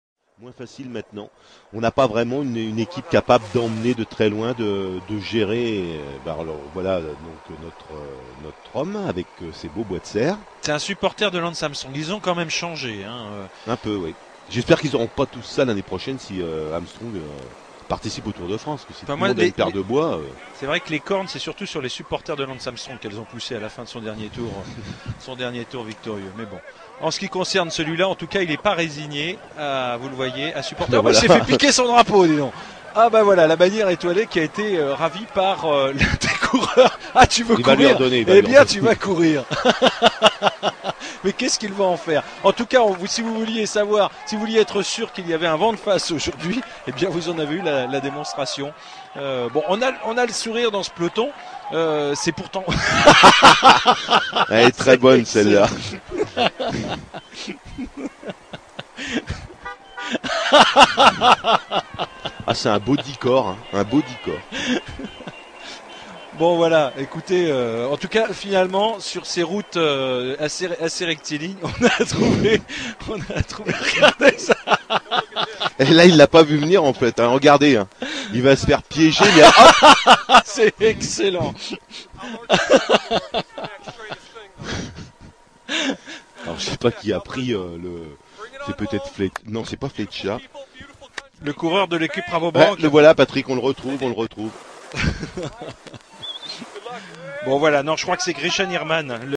Juan Antonio Flecha steals American flag of a moose in Vuelta 2008
Juan Antionio Flecha 4 president!